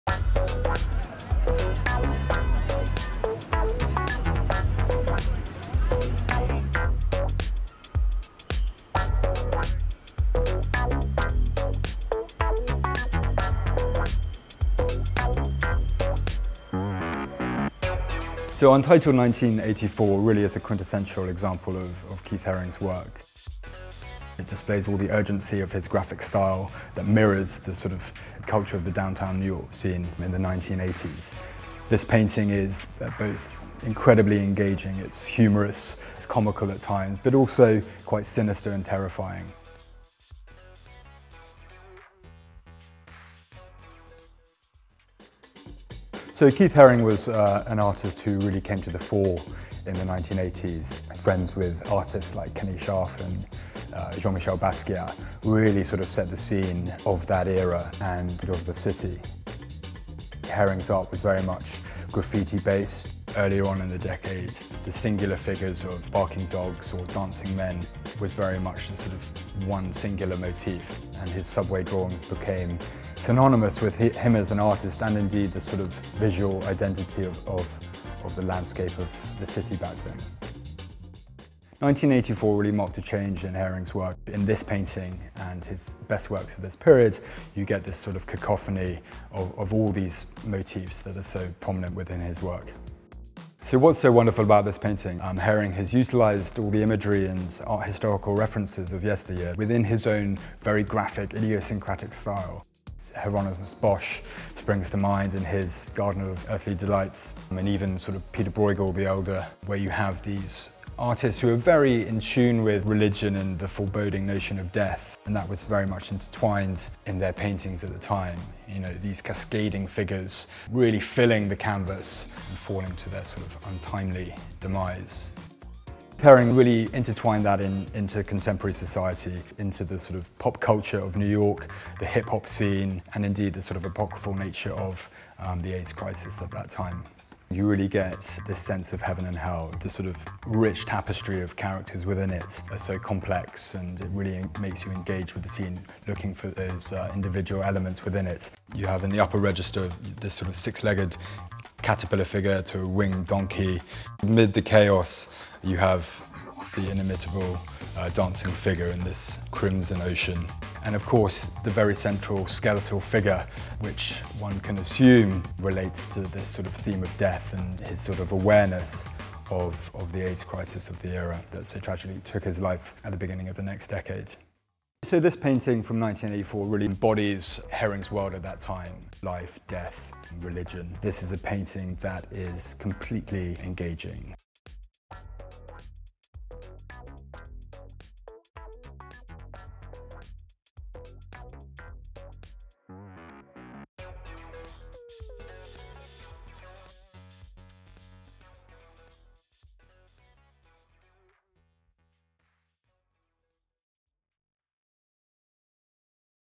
Keith Haring’s Nightmarish Vision
Against a backdrop of acid yellow, a teaming mass of fantastical beasts and hybrid creatures inhabit Keith Haring’s ‘Untitled’ from 1984. In this video, discover how Haring began to explore themes of mortality as he found himself at the centre of a community ravaged by AIDS. Learn also how Haring’s apocalyptic vision of heaven and hell invites comparison with the works of Pieter Brueghel the Elder and Hieronymus Bosch, updated with the dancing B-Boys of early 1980s New York’s graffiti and hip-hop scene. ‘Untitled’ is a highlight of our upcoming cross-category ‘Rembrandt to Richter’ Evening Sale (28 July | London).